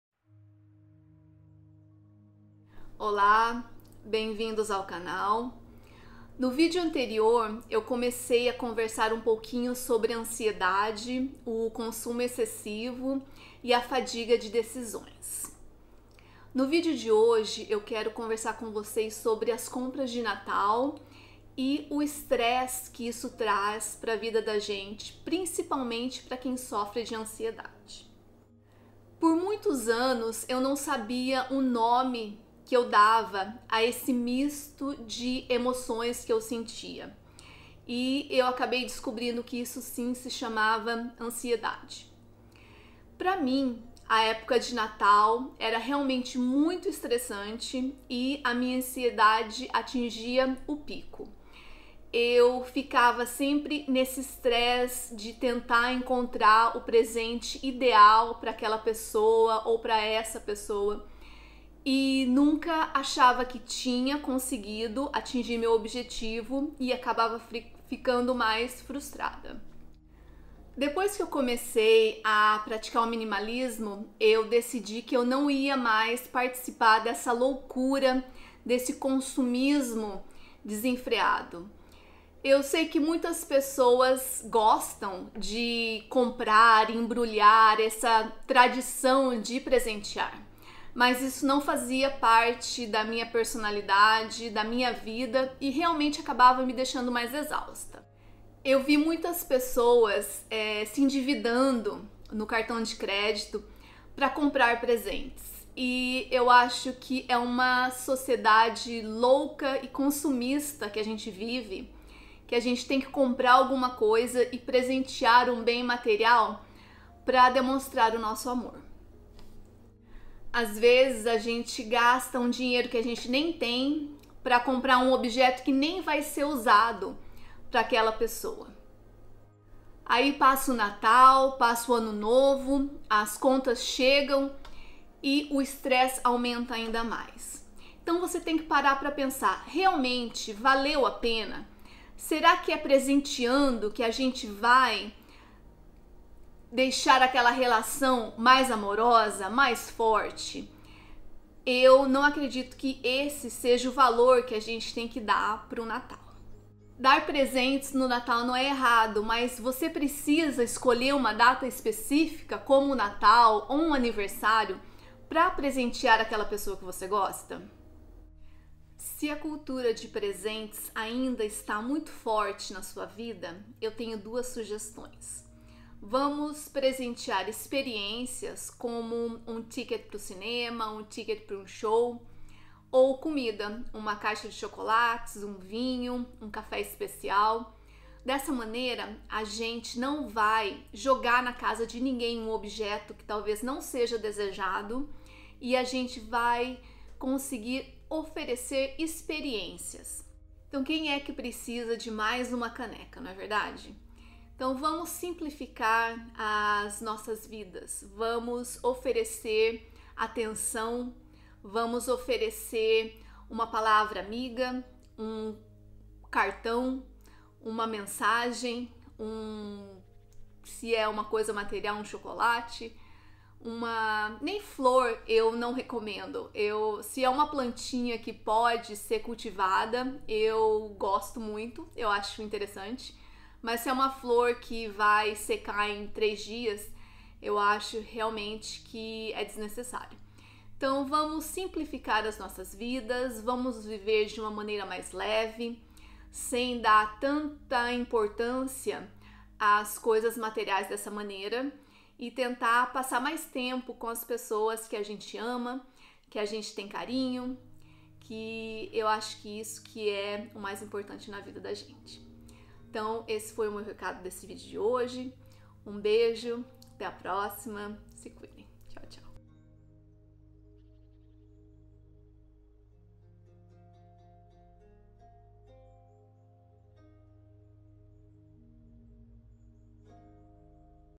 COMO O MINIMALISMO PODE NOS AJUDAR A CONTROLAR A ANSIEDADE NO NATAL.
Bem-vindos. No vídeo de hoje eu converso com vocês sobre as compras de natal e porque essa época do ano é tão estressante para quem tem ansiedade. Muitas vezes tomar decisões pode trazer uma exaustão mental e decidir não participar desse ritual de comprar presentes pode ser uma boa opção e pode noz trazer uma paz incrível. A gente tem que se escolher e se deixar de dar presentes não é uma opção agora pra você, uma dica é dar gift cards, porque o gift card vai tirar de você a pressão de ter que escolher, o que muitas vezes aumenta a nossa ansiedade. Um beijo, se cuidem 🌸